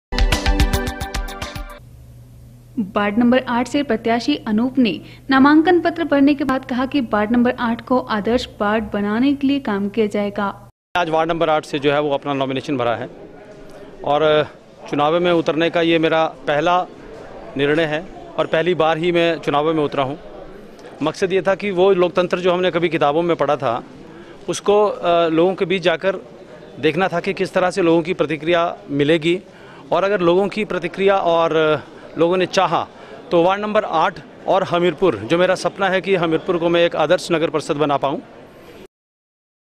28 DEC 19 अनूप ने नामांकन भरने के बाद कहा कि वार्ड को आदर्ष वार्ड बनाने के लिए काम किया जाएगा।
वार्ड नंबर आठ से प्रत्याषी अनूप ने नामांकन पत्र भरने के बाद कहा कि वार्ड नंबर आठ को आदर्ष वार्ड बनाने के लिए काम किया जाएगा।